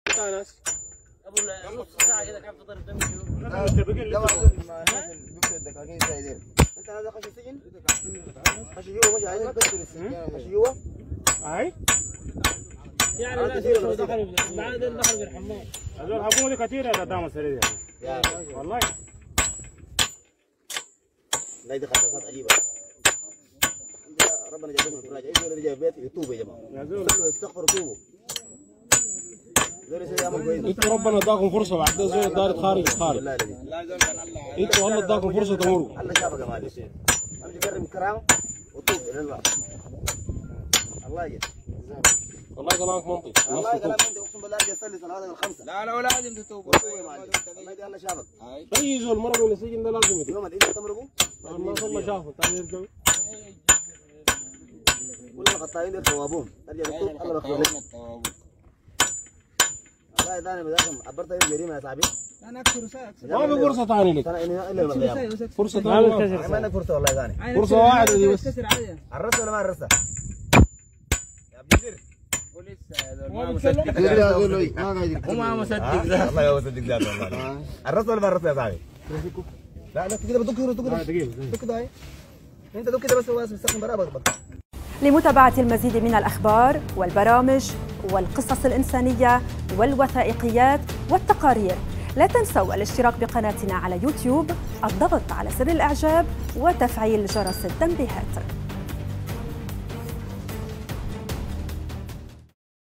سجين فار من سجن الهدى يحاول التخلص من قيوده
شاهد.. فيديو متداول لسجين فار من سجن_الهدى يحاول التخلص من قيوده